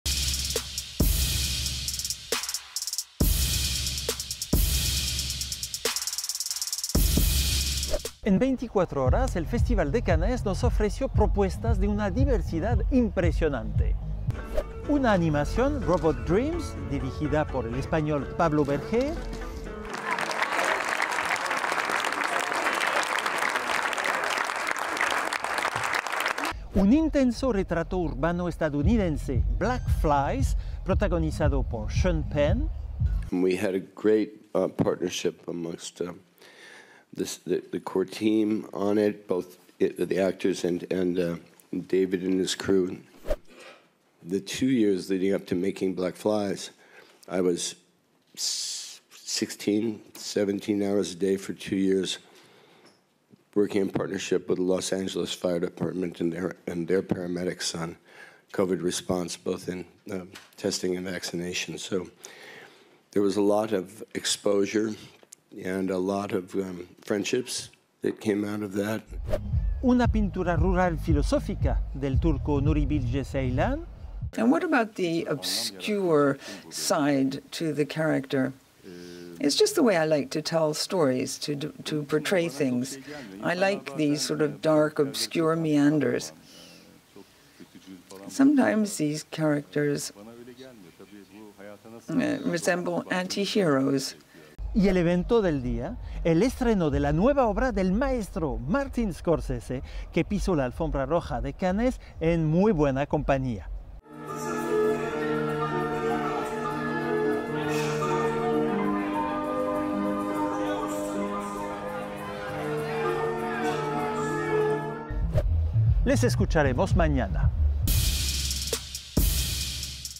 Festival de Cine de Cannes 2023 con Jean-Christophe Berjon | Capítulo 5
El actor, crítico y dramaturgo Jean-Christophe Berjon presenta un resumen de lo más destacado del Festival de Cine de Cannes 2023. En esta cápsula se hace un reconocimiento a la diversidad tanto en géneros y temáticas, como en técnicas cinematográficas presentada a lo largo de esta celebración.